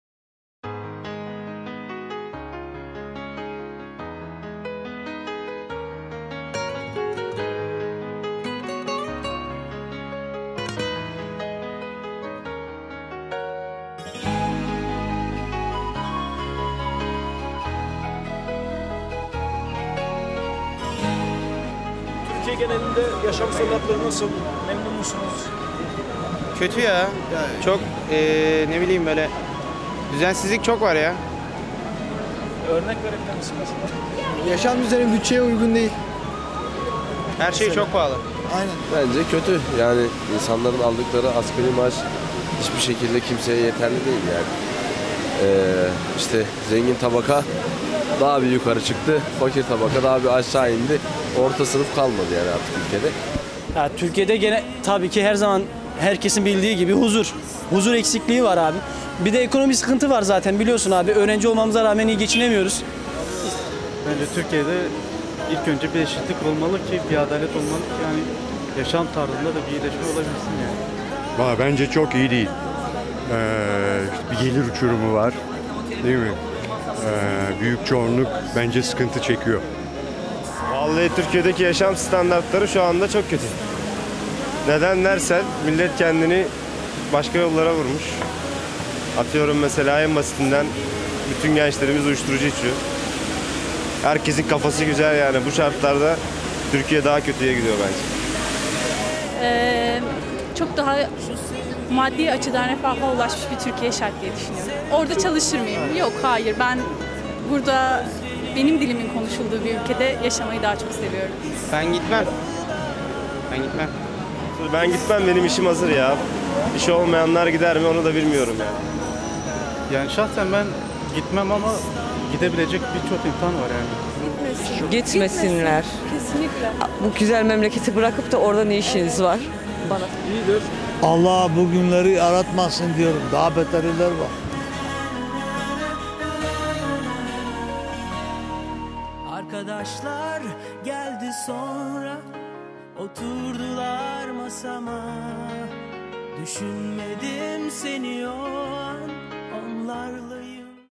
Türk gençleri Avrupa'da çalışmak istermi? - Sürpriz Son (İstanbul 2016)
Merhaba Arkadaşlar,

kısa bir süreliğine İstanbul'daydım.. kendimi en iyi hissetdığım ülkede.. MEMLEKETİMDE..
Sokağa çıkıp yurtdaşlarımın hayatlarından memnun olup olmadıklarını sormak istedim.. ardından Avrupa'da yaşamak isteyip istemediklerini...

kayıt altına aldığım röportajları inceledikten sonra, kendime şu soruyu sormakdan alıkoyamadım...

Hayatın getirdiği zorluklar NE OLURSA OLSUN..
Acaba biz Hayata daha umutlu ve pozitif baksak, Hayatımızda bi değişiklik olurmu? Yoksa gerçekten Hayat bu kadar zormu?

Keyifli seyirler